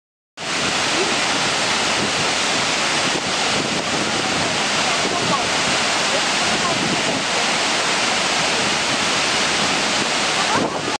銚子大滝　今日は　格別に水量が多いです　Ｈ26.07.27.